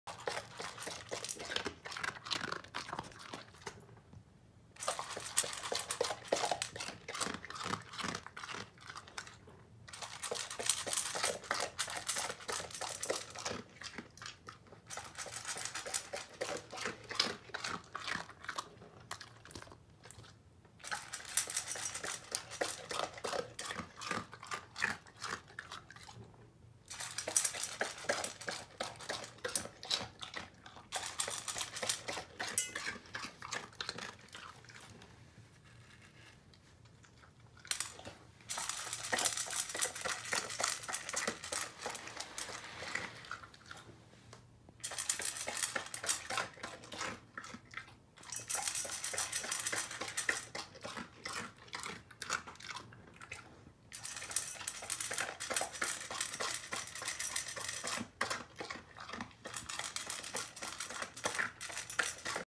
asmr 勉強中♬
愛犬ユイちゃんのカリカリ音♬